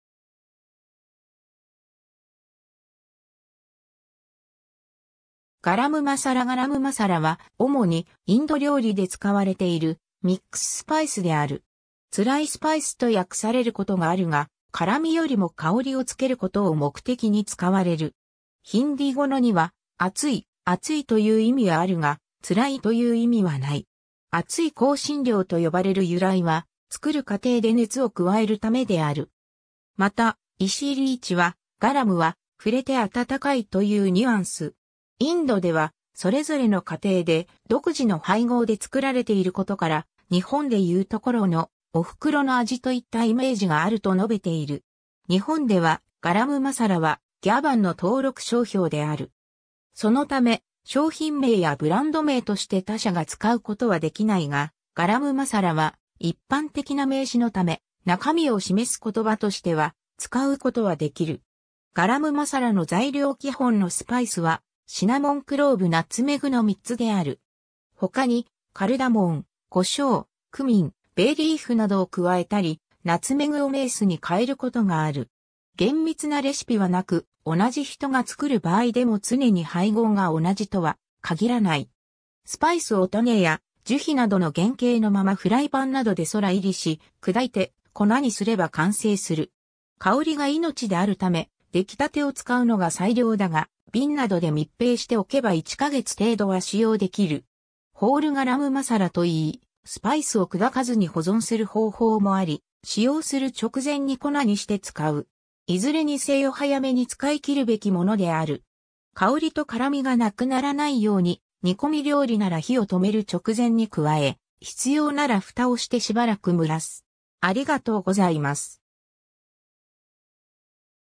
ガラムマサラ
ガラムマサラ ガラムマサラ（ガラムマサーラー、 garam masala ; گرم مسالا ; गरम मसाला ）は、主にインド料理で使われているミックススパイス（香辛料）である。
 「辛いスパイス」と訳されることがあるが、辛味よりも香りをつけることを目的に使われる。
ヒンディー語の "गरम" には「暑い」「熱い」という意味はあるが「辛い」という意味はない。
 熱い香辛料と呼ばれる由来は、作る過程で熱を加えるためである（後述）。
また、石井利一（S&Bスパイスクッキングアドバイザー）は、「『ガラム』は、“触れてあたたかい”というニュアンス。インドでは、それぞれの家庭で独自の配合で作られていることから、日本でいうところの『おふくろの味』といったイメージがある」と述べている。
 日本ではガラムマサラはギャバンの登録商標(第1586997号、区分はスパイス類など)である。
そのため、商品名やブランド名として他社が使う事はできないが、「ガラムマサラ」は一般的な名詞のため中身を示す言葉としては使う事は出来る。
 ガラムマサラの材料 基本のスパイスは、 シナモン（肉桂、桂皮）・クローブ（丁子）・ナツメグ（肉荳蔲）の3つである。
 ほかにカルダモン、胡椒、クミン、ベイリーフなどを加えたり、ナツメグをメースに替えることがある。
厳密なレシピは無く、同じ人が作る場合でもつねに配合が同じとは限らない。
 スパイスを種や樹皮などの原型のままフライパンなどで空煎りし、砕いて粉にすれば完成する。
香りが命であるため出来立てを使うのが最良だが、瓶などで密閉しておけば1カ月程度は使用できる。
「ホール・ガラムマサラ」といい、スパイスを砕かずに保存する方法もあり、使用する直前に粉にして使う。
いずれにせよ早めに使いきるべきものである。
 香りと辛味が無くならないように、煮込み料理なら火を止める直前に加え、必要なら蓋をしてしばらく蒸らす。